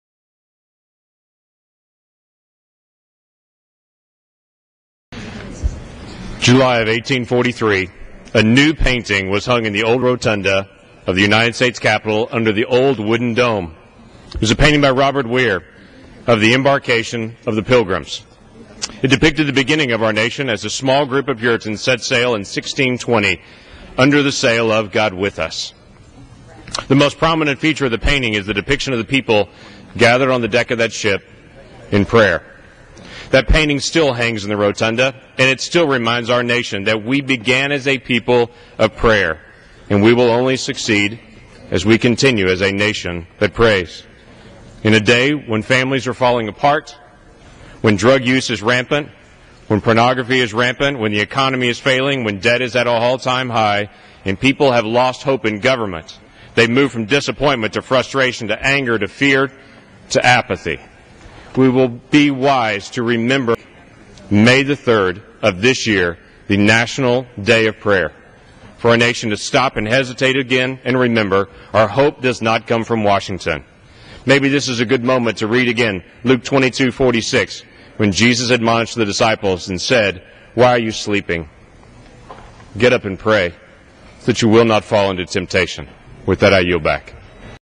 Rep. James Lankford Remembers the National Day of Prayer
Rep. James Lankford (R-OK) remembered the National Day of Prayer on the House floor. (April 27, 2012)